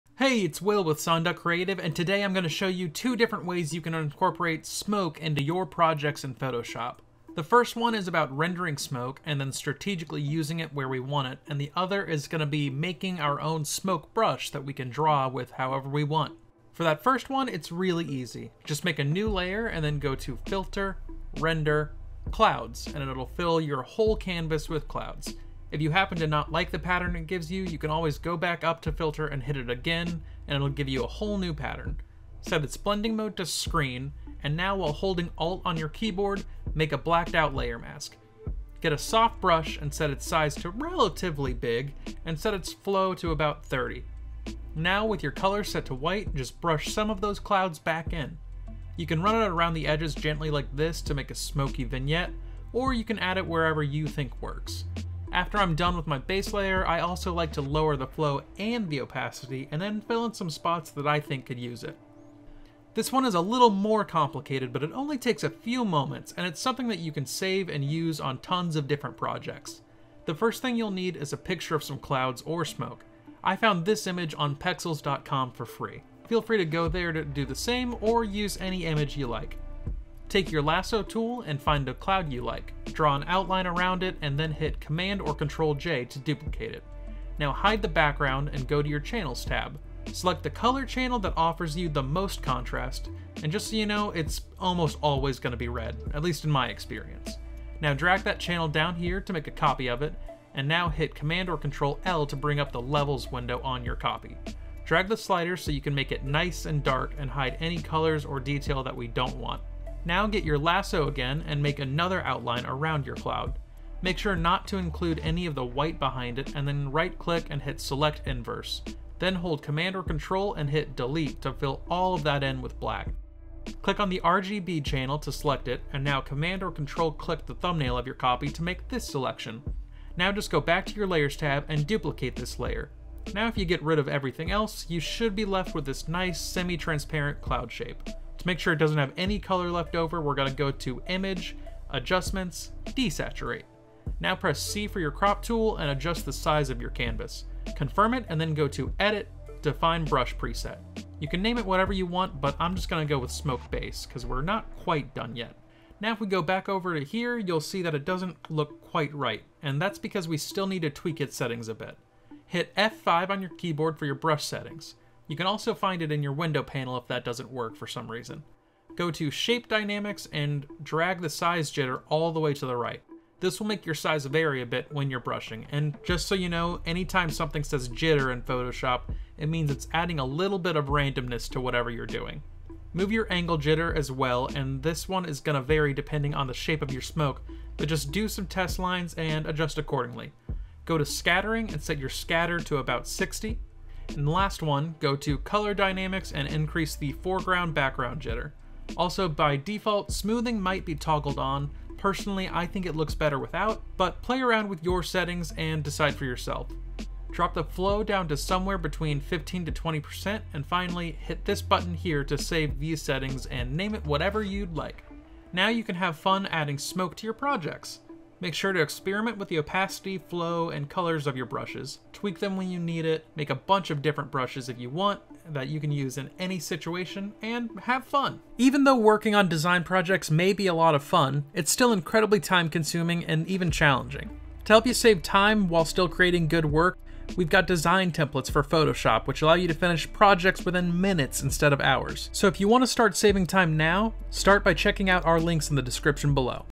Add Smoke Effects For Photos in Adobe Photoshop | Tutorial
Learn how to create stunning smoke effects for your photos using Adobe Photoshop. In this tutorial, we'll show you step-by-step how to use filters and blend modes to create realistic smoke in just a few minutes. Whether you're a professional photographer or just a hobbyist, this tutorial will help you take your photography to the next level. Watch the video now and start creating amazing smoke effects for your photos!

► Get access to over a million Photoshop templates and other assets:
1.envato.market/15jAPD